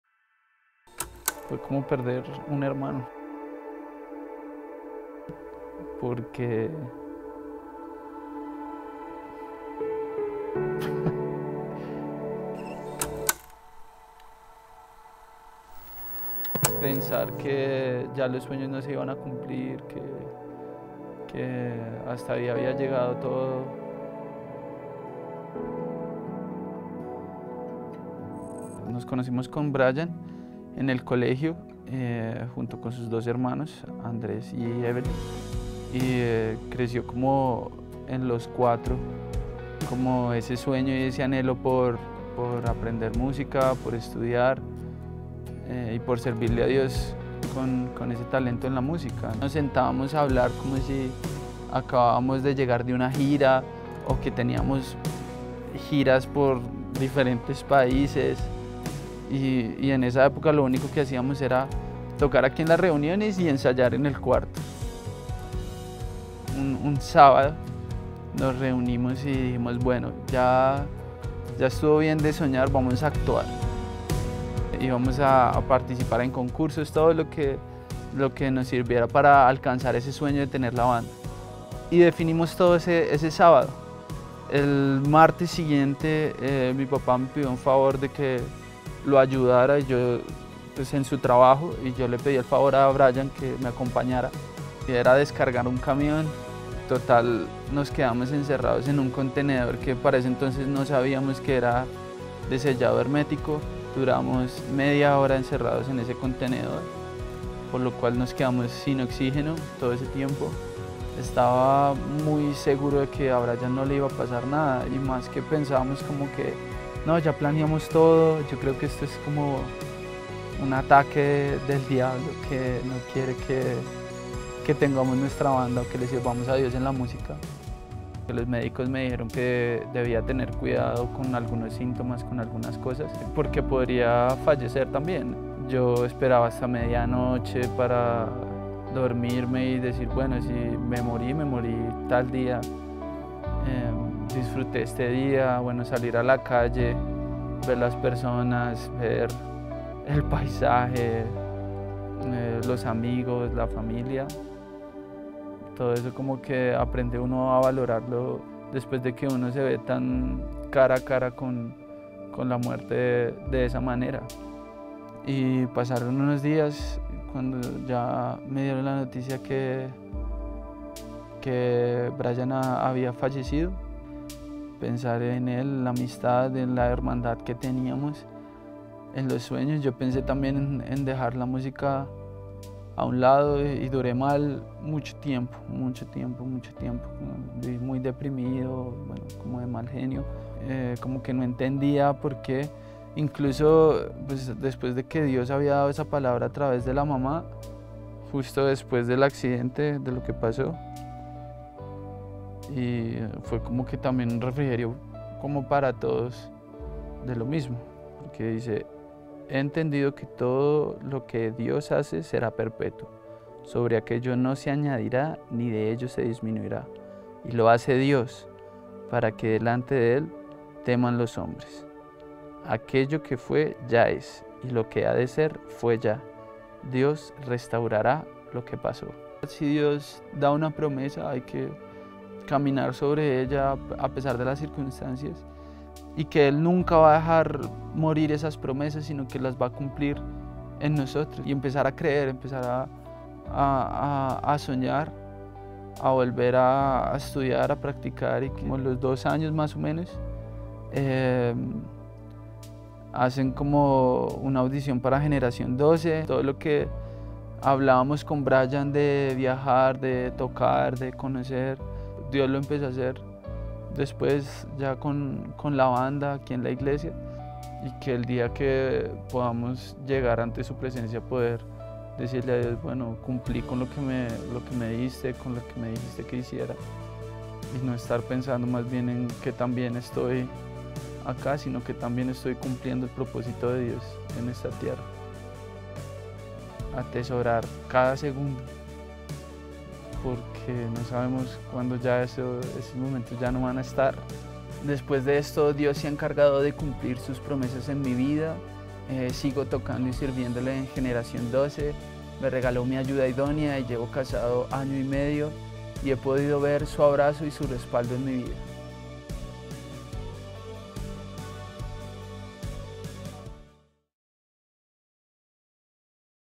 Enseñanza para células - Tu abrazo otra vez
Testimonio que hace parte del kit para células "Tu luz brilla"
Deseas tener este kit?